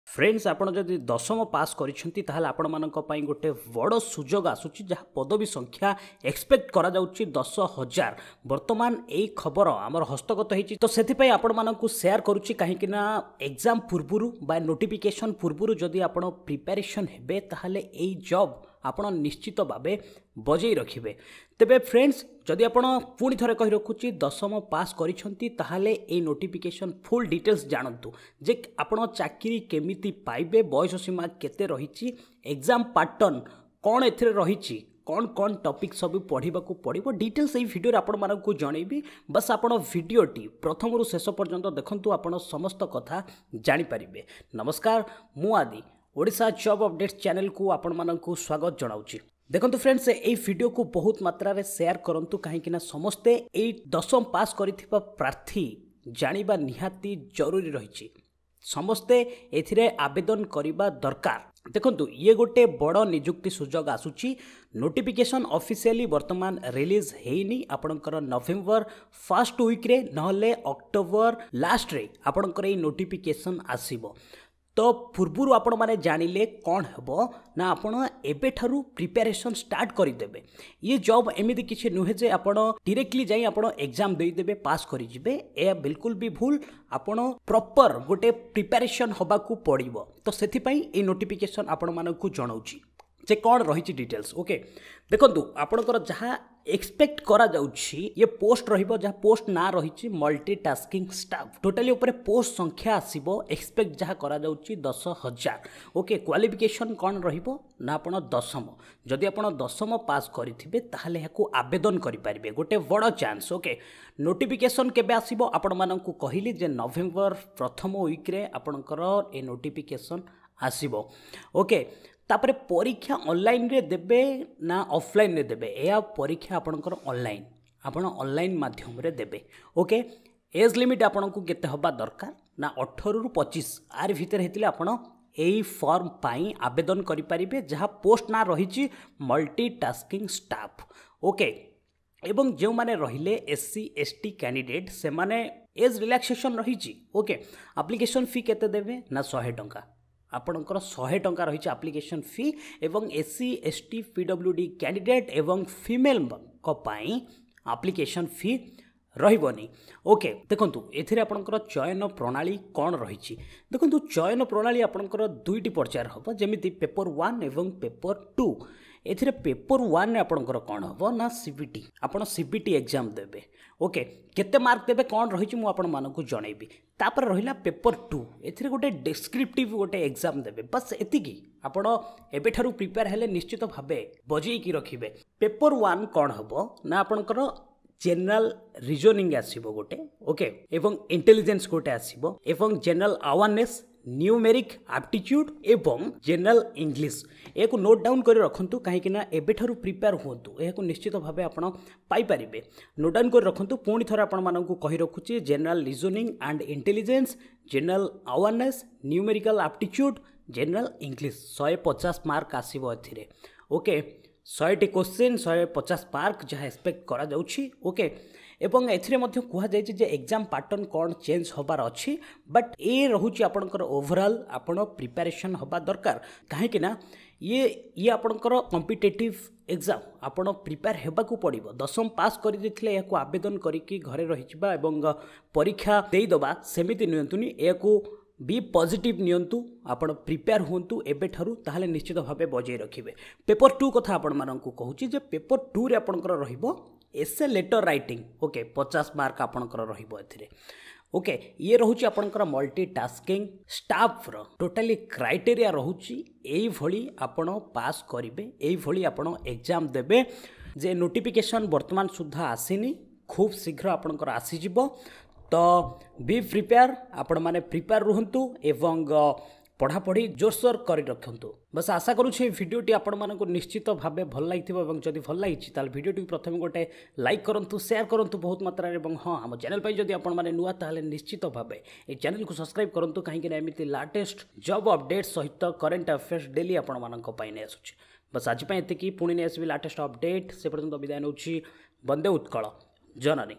10000 MTS posts for Matric pass Candidates | Salary Rs.21000 | Know more
All about MTS Recruitment notification 2018
Expected total posts - 10,000
Post name - Multi Tasking Staff
Qualification - Matric pass
Salary - Rs.21,000 +
Upcoming Job

About : Odisha Job Updates is a YouTube Channel, where you will find all kind of job information & Educational video in Odia, New  video is posted everyday.